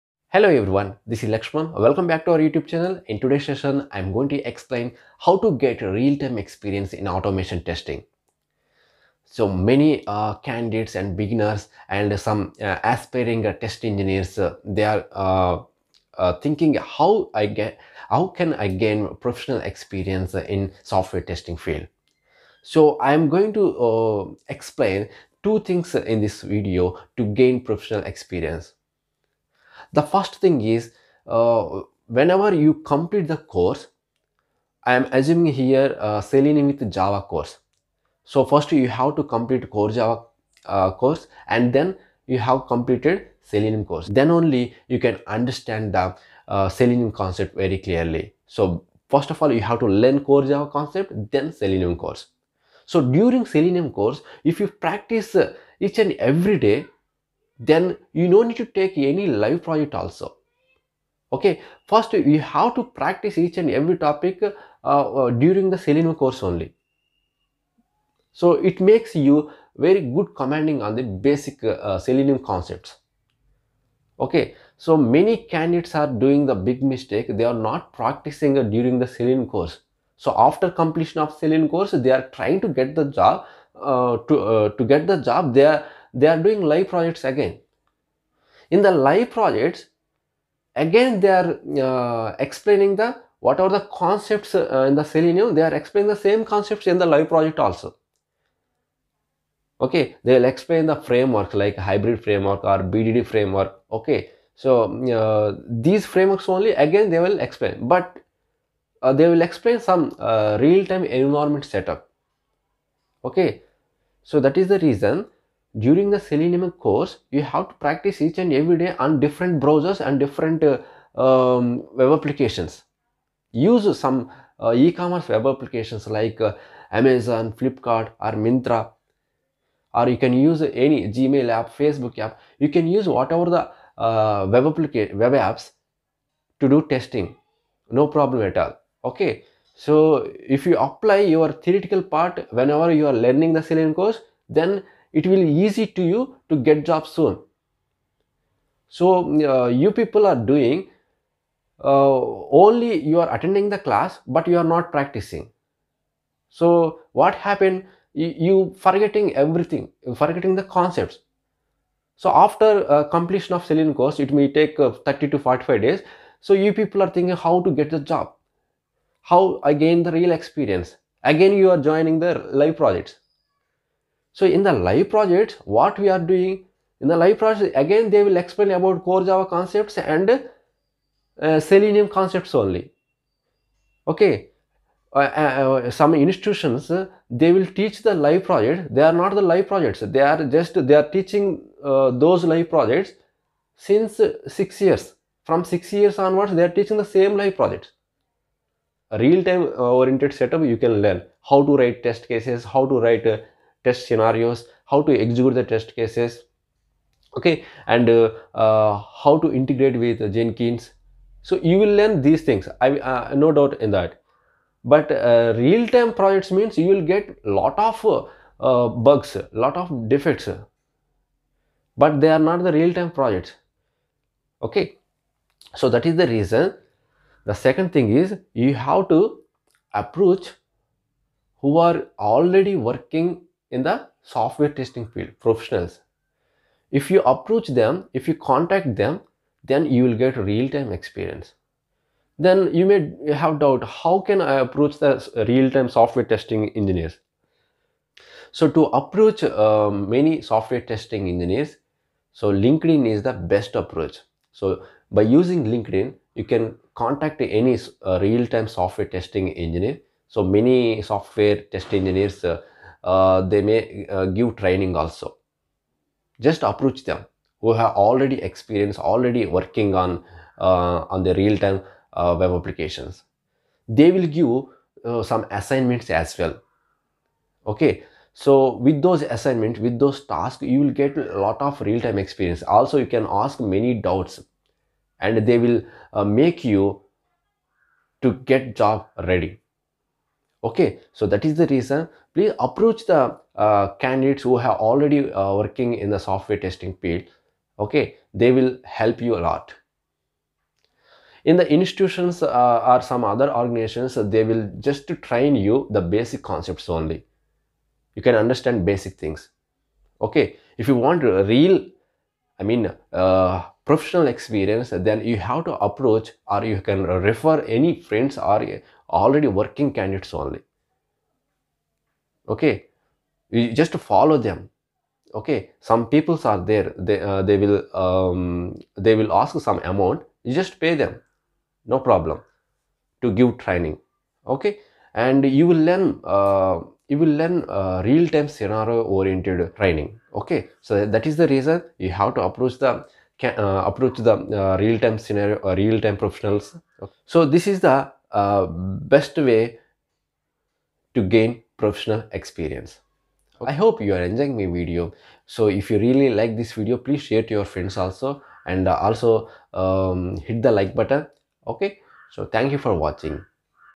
How to Get Real time Experience in Software Testing
Hi Friends!!! welcome to @byluckysir . In this video you will know how to gain professional experience in automation Testing or we can say how to get real time experience in Software Testing.

For more videos visit playlist and watch and enjoy the videos